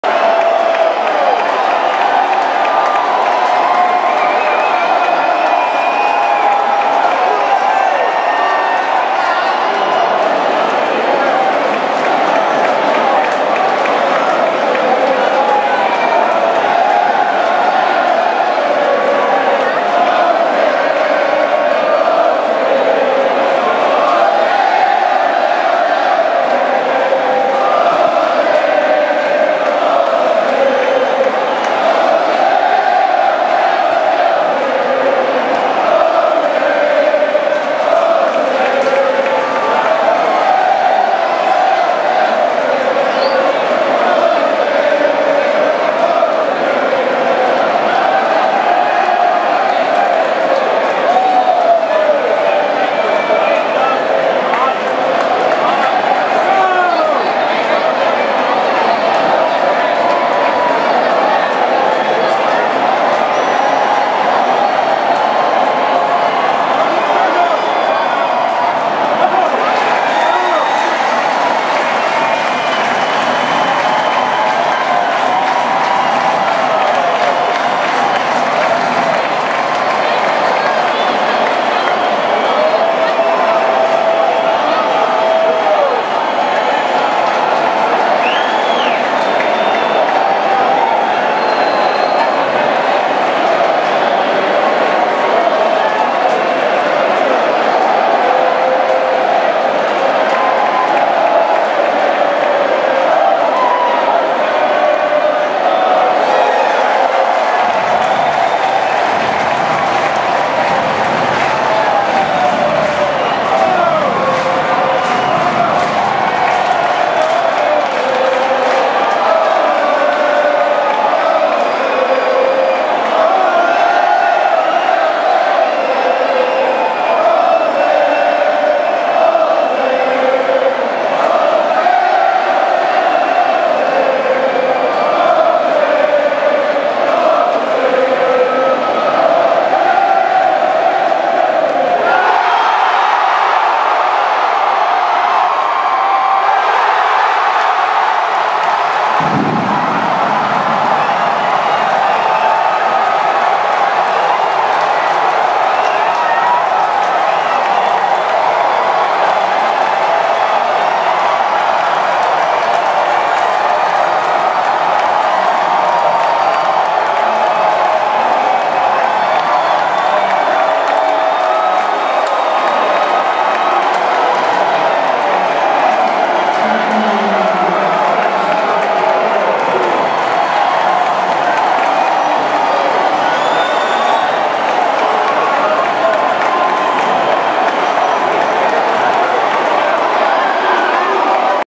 Jose Altuve at bat: home run...ALDS Game 5...Astros vs. Rays...10/10/19
Jose Altuve homers against Emilio Pagan of the Tampa Bay Rays in the 8th inning of ALDS Game 5 at Minute Maid Park in Houston.